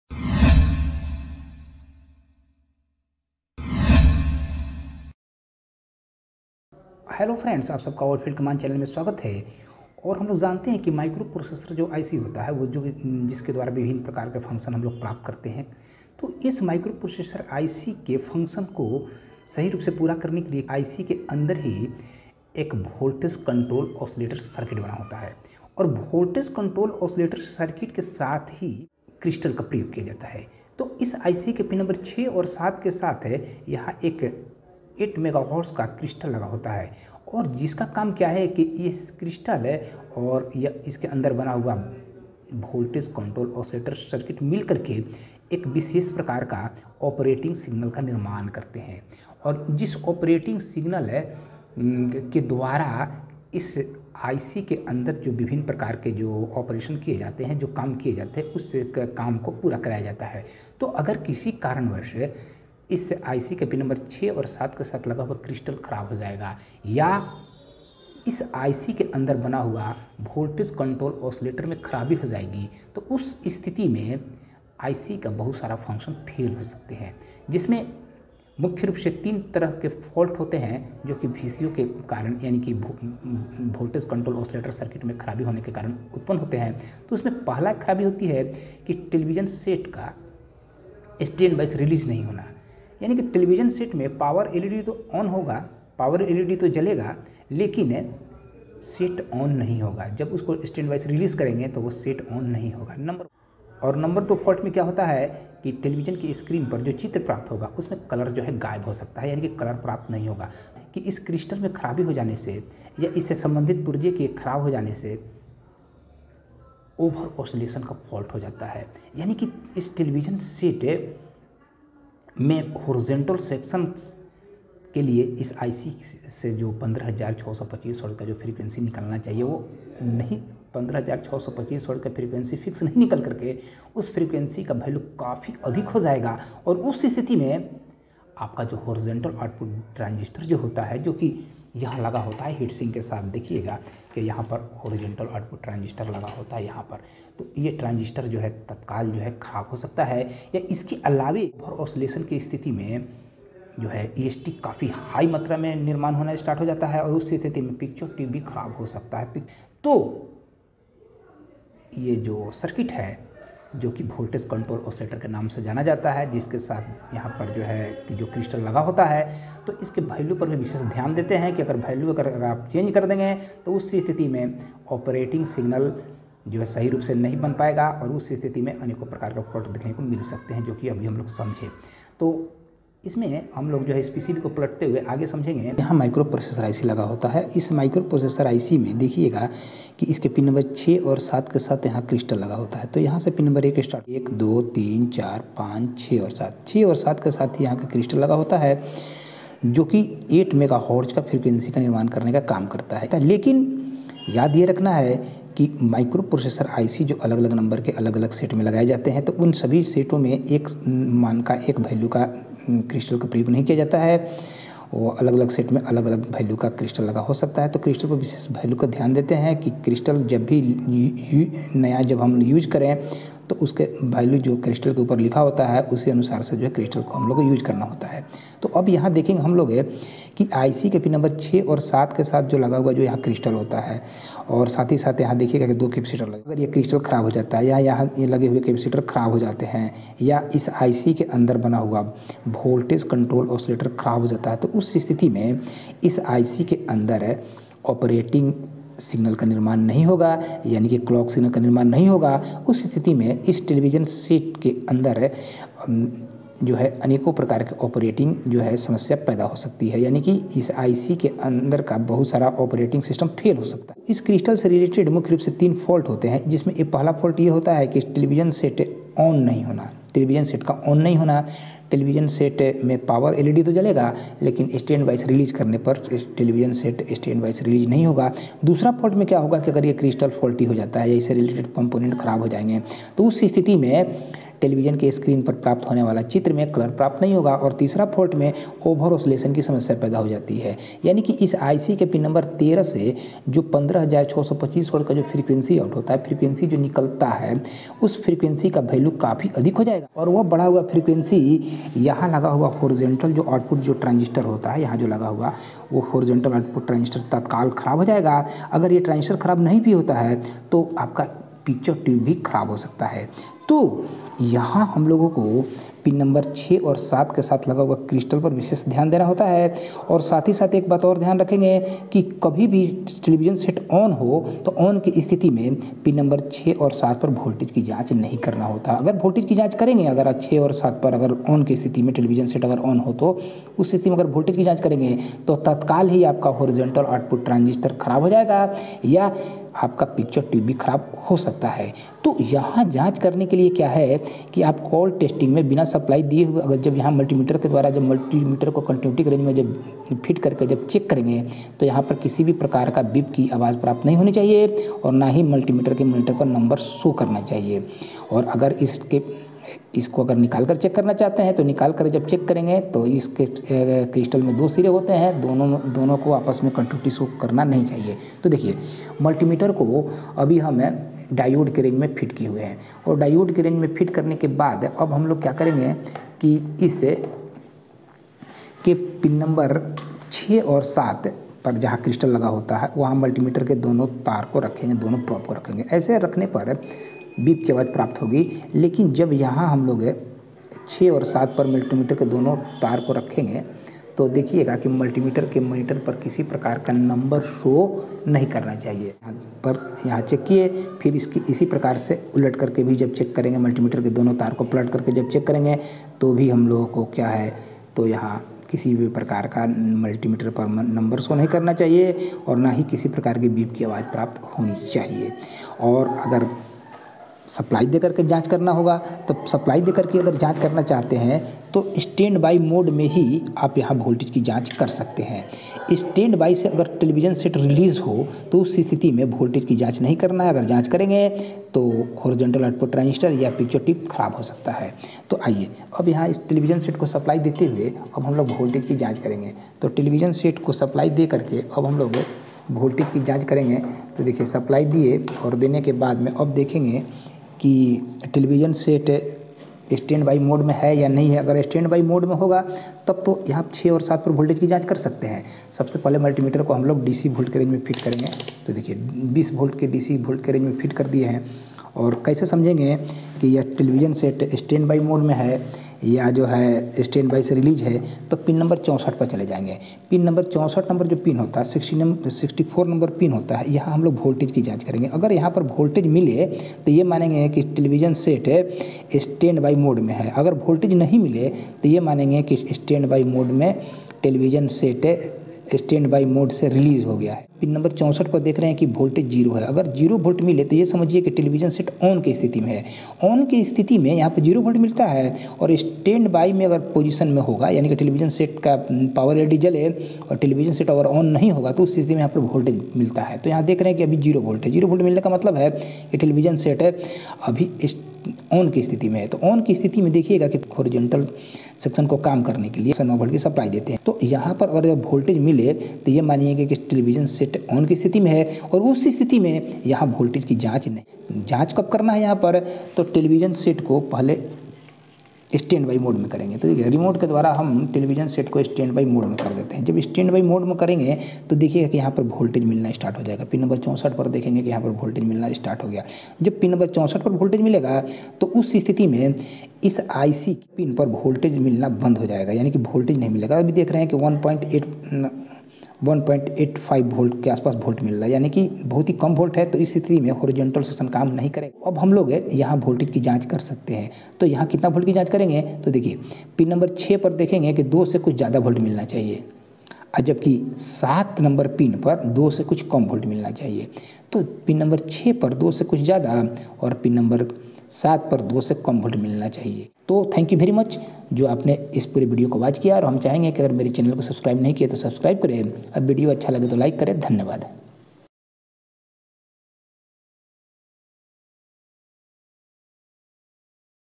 V.C.O / Crystal का क्या काम होता हैं।
Voltage control oscillator is the most important circuit of the C.R.T television
Voltage control oscillator (v c o) play very important role in the standby section